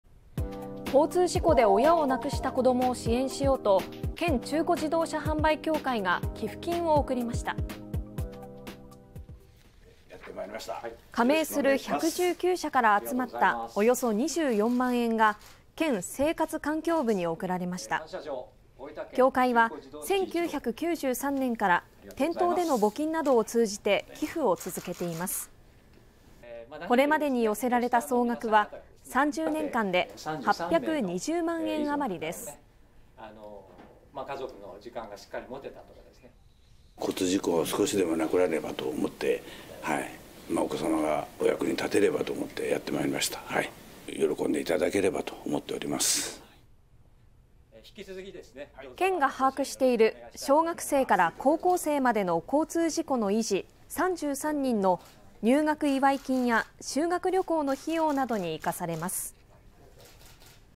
【大分】中古車販売経協会が交通事故遺児を支援
交通事故で親を亡くした子どもを支援しようと、大分県中古自動車販売協会が寄付金を贈りました。